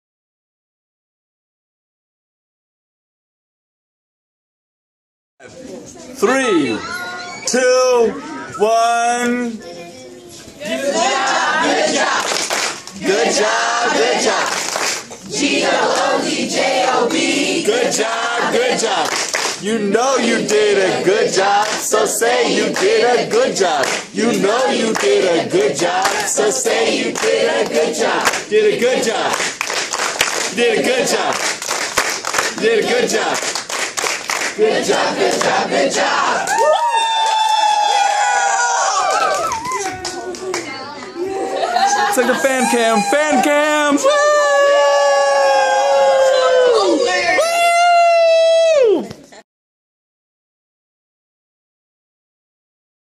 15for15 Good Job, Good Job
We did it!  We reached our 15 Year campaign goal.  Thank you for helping us serve more NYC Youth.  For more info on the campaign and Kids Creative,